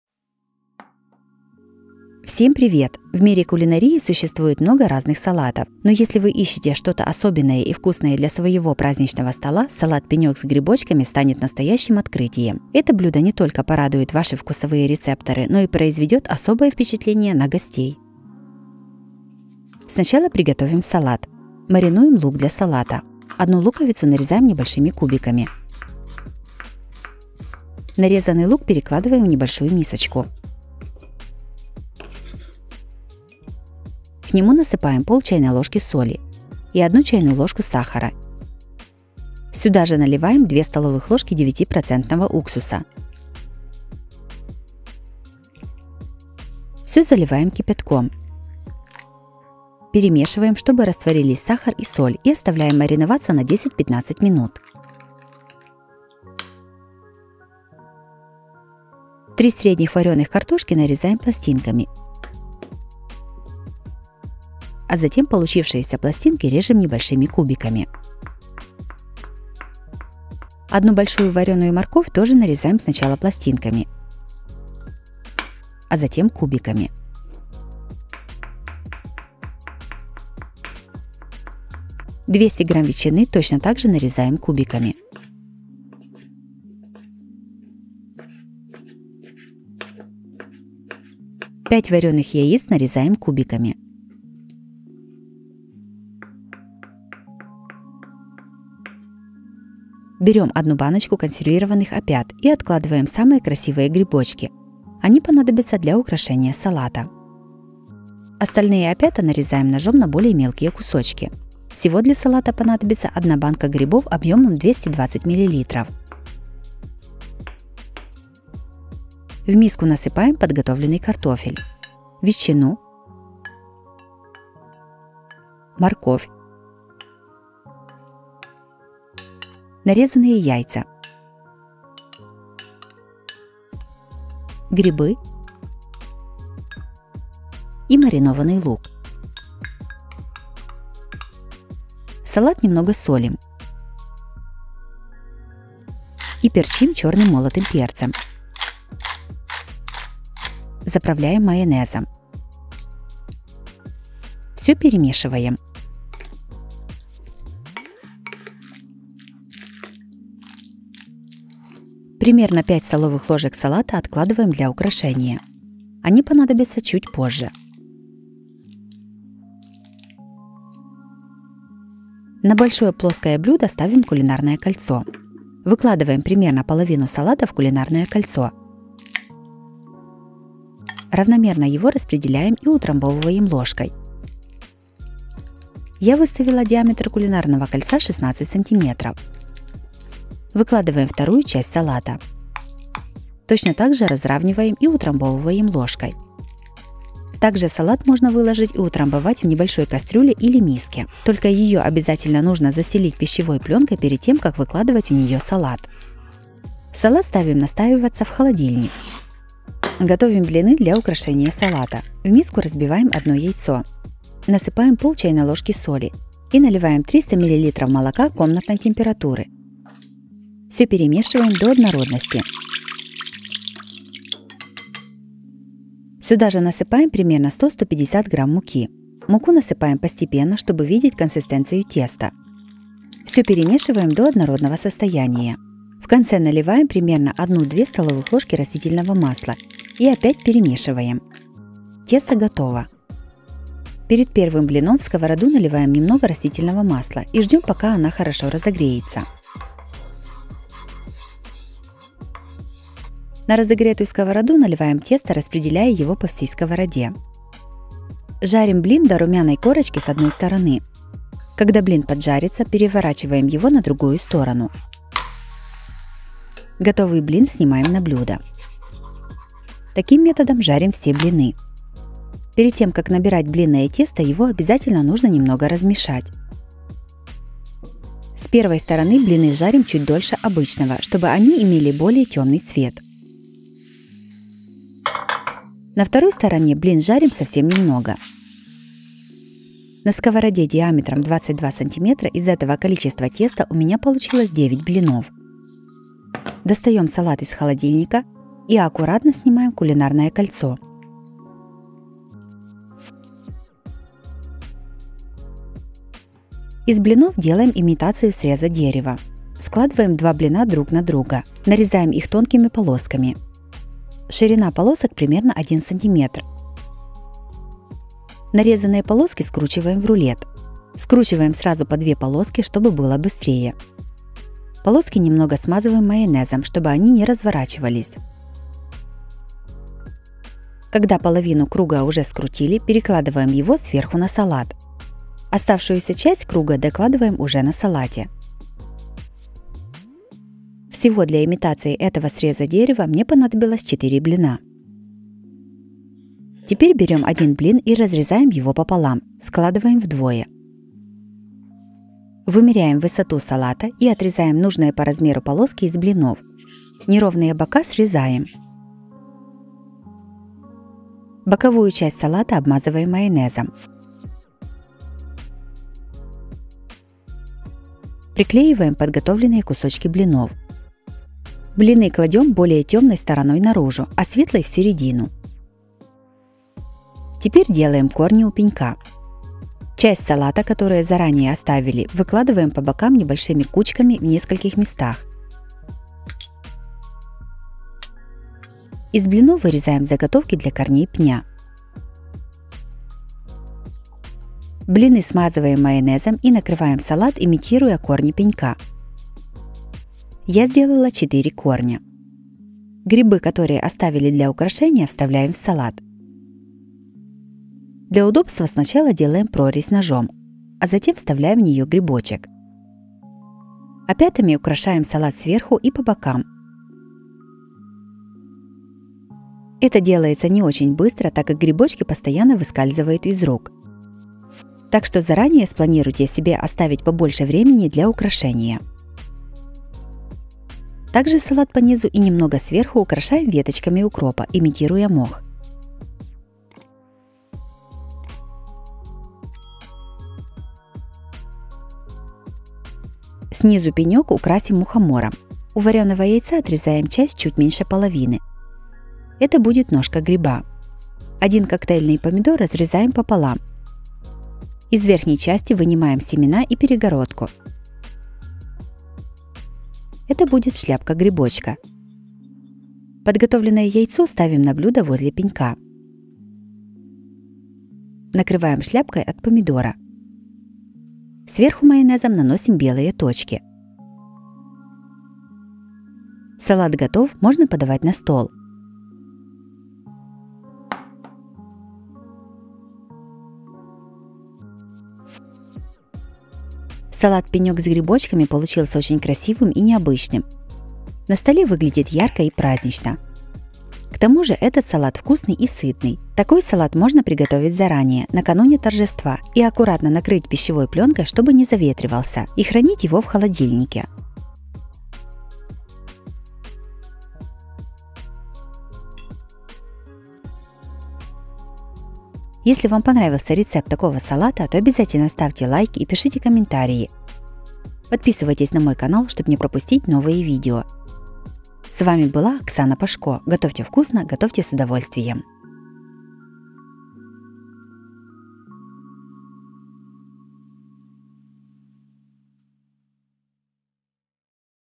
МЫ просто ОБОЖАЕМ ЭТОТ САЛАТ! Безумно вкусный и красивый!
Салат «Пенек с грибочками» станет настоящим открытием. Мы просто обожаем этот салат! Безумно вкусный и красивый! Это блюдо не только порадует ваши вкусовые рецепторы, но и произведет особое впечатление на гостей.

ИНГРЕДИЕНТЫ:
картофель - 3 шт.
морковь большая - 1 шт.
опята маринованные - 1 банка (220 мл)
соль
перец черный молотый
Маринад:
горячая вода - 150 мл
Блины:
Украшение:
укроп
яйца - 1 шт.
помидоры коктейльные - 1 шт.